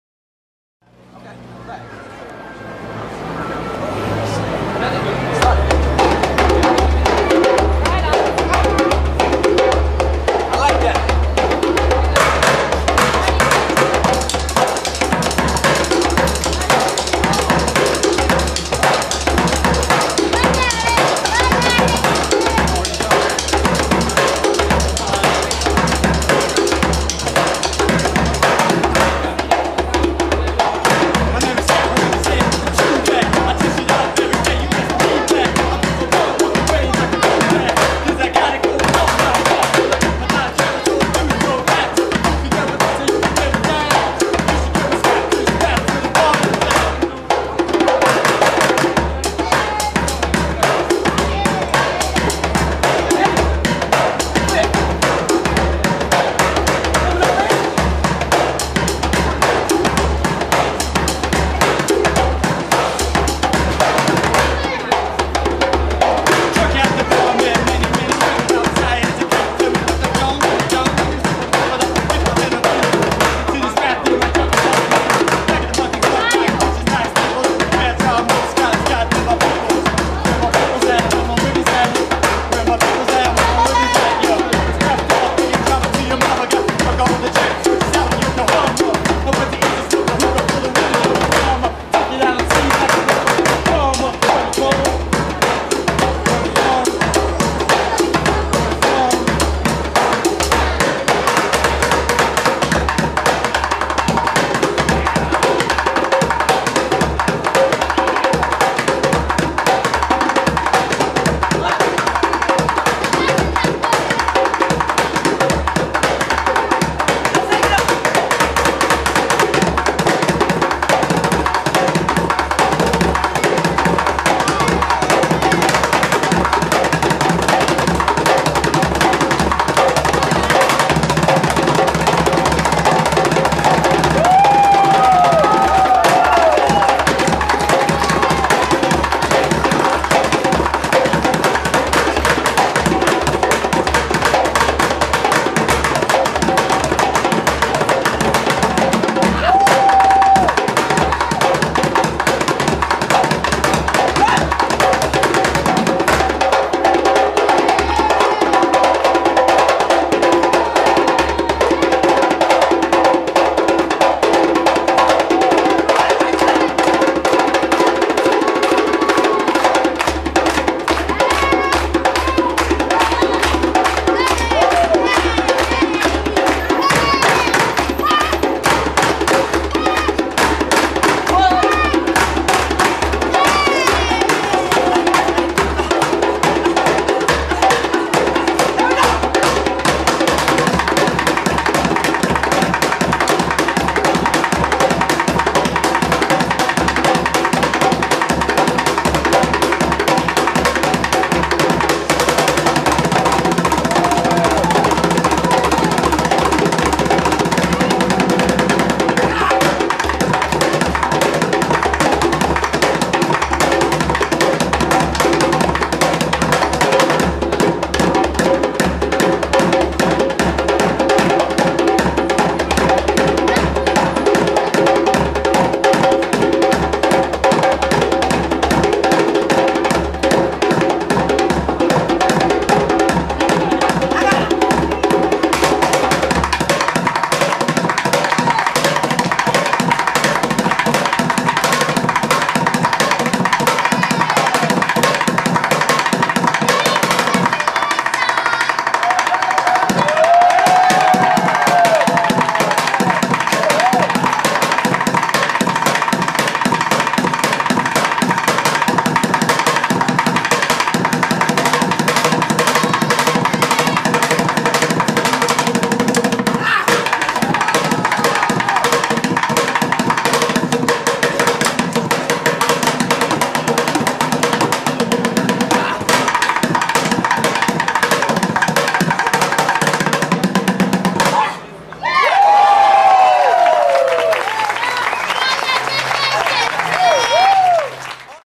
Scrap Star and Larry Wright drum street performance
Cruising through New York with my djembe drum, I came across the famous bucket drummer Larry Wright so we did an improv jam session.